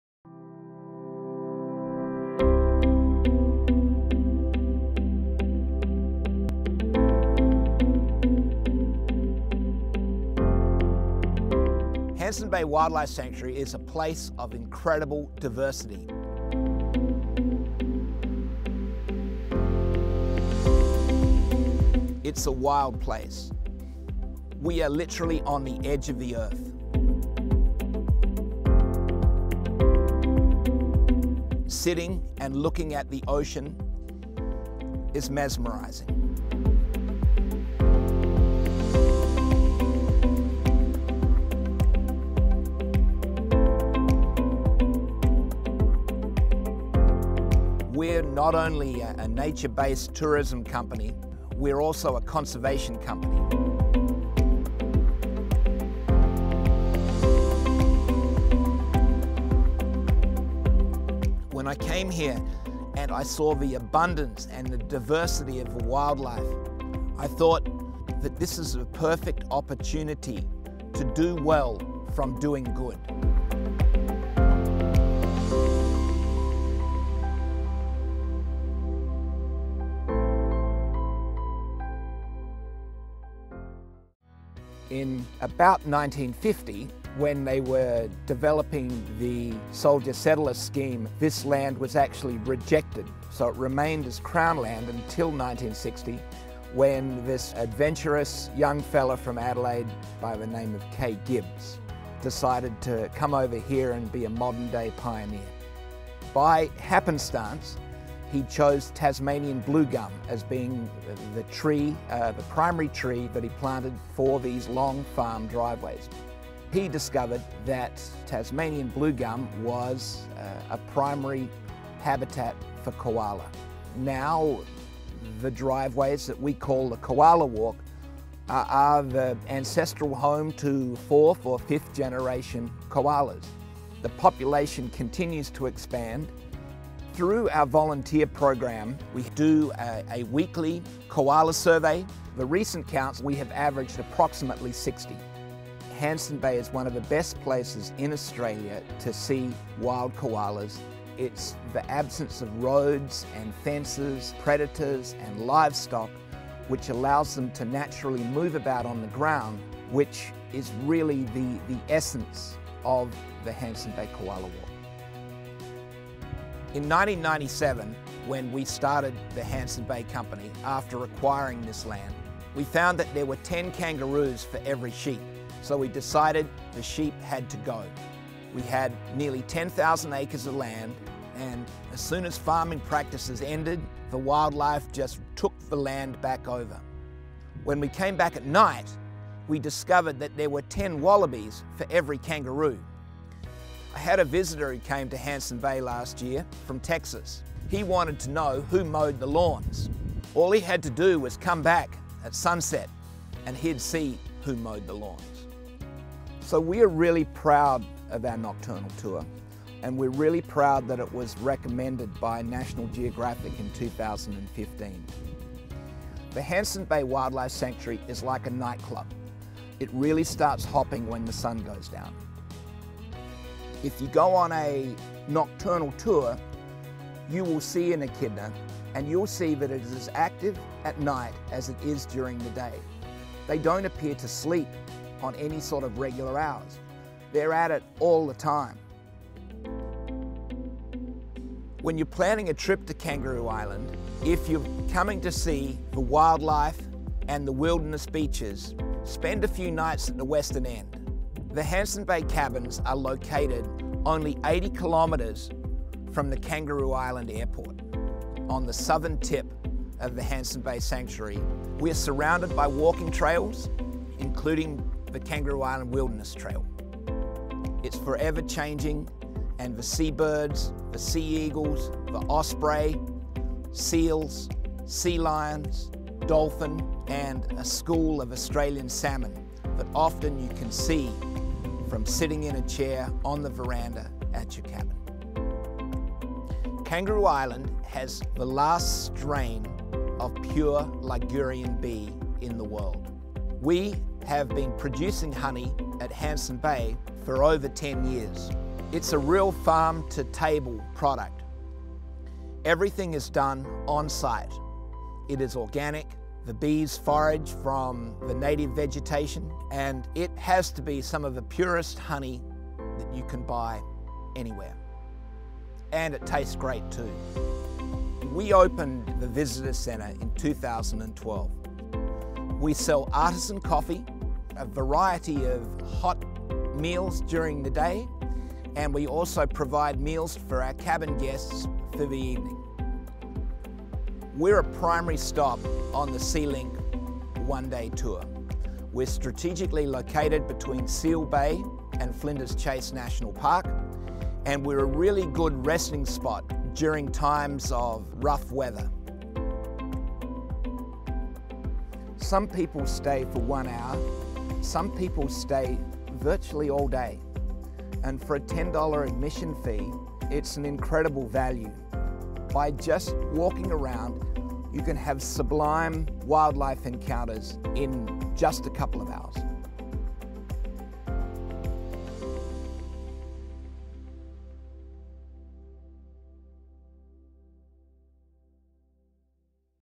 Hanson Bay Wildlife Sanctuary
Short promotional video for the Hanson Bay Wildlife Sanctuary on Kangaroo Island. Wildlife, wild coasts and wild adventure; a stunning place to visit and stay.